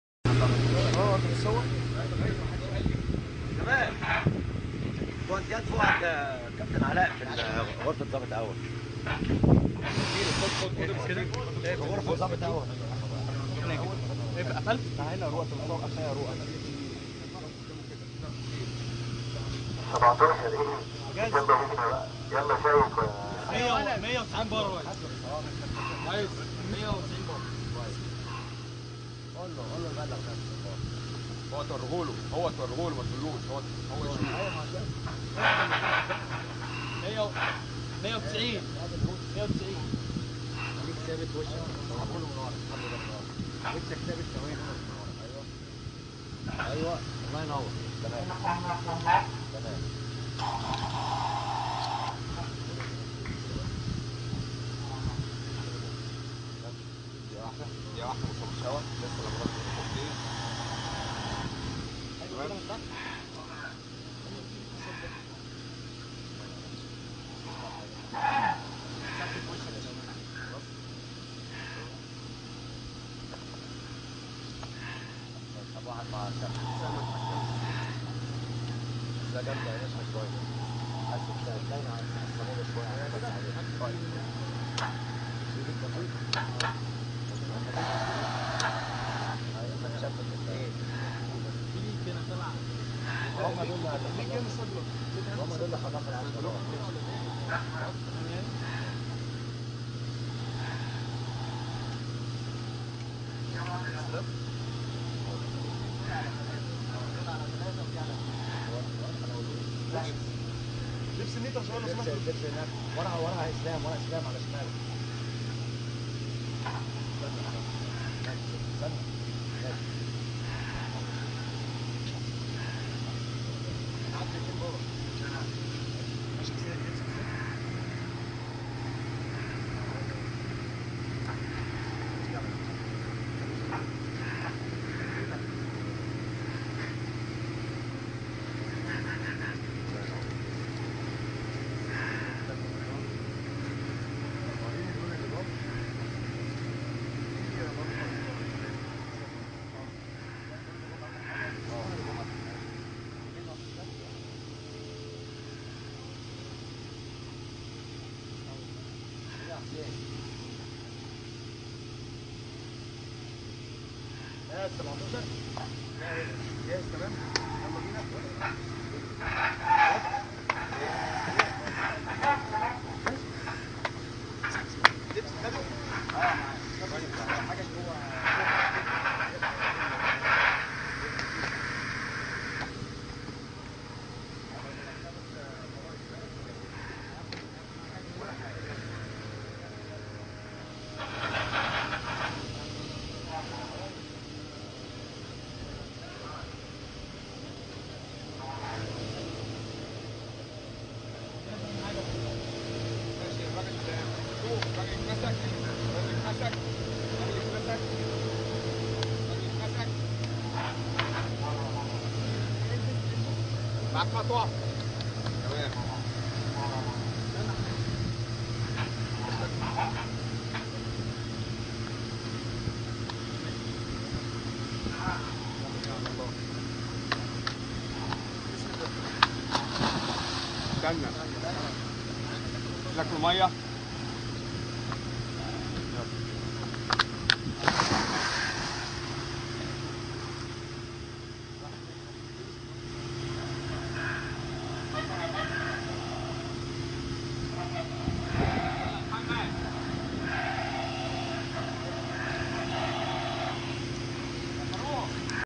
prepar main diver Hussein to dive to install gas flange clamp in Egypt
part from 37m under water dive in porfouad gas field in Egypt  team (Hussein the main diver -me- Kmal second diver, capten Alla Azamy and Eng Islam Elgritly supervisor , Saeed Sabry , Elbadry , Mrwan , Shady ,  Mohammed Farouk , Elgohary , and Gmal